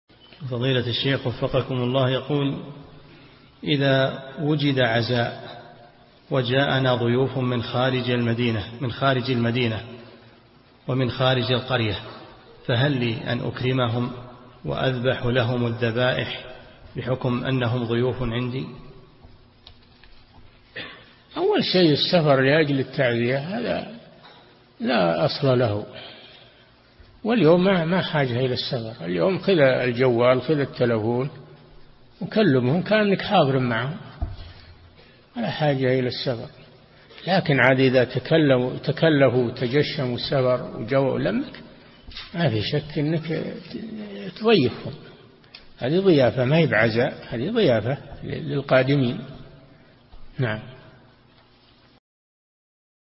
[491 -939] إذا وجد عزاء وجاءنا ضيوف من خارج المدينة فهل لي أن أكرمهم وأذبح لهم الذبائح؟
-[491 -939] السؤال: إذا وجد عزاء وجاءنا ضيوف من خارج المدينة ومن خارج القرية فهل لي أن أكرمهم وأذبح لهم الذبائح بحكم أنهم ضيوف عندي ؟
الجواب: أول شيء السفر لأجل التعزية هذا لا أصل له، واليوم ما حاجة إلى السفر خذ الجوال خذ التلفون وكلمهم كأنك حاضر معهم فلا حاجة إلى السفر لكن إذا تكلفوا  وتجشموا السفر وجاءوا ما في شك أنك تضيفهم هذه ضيافة ما هي بعزاء هذه ضيافة لأجل للقادمين.
للاشتراك في قناة الشيخ صالح الفوزان

للاستفسار والإقتراحات والدعم
رئيس الفريق
لمشروع كبار العلماء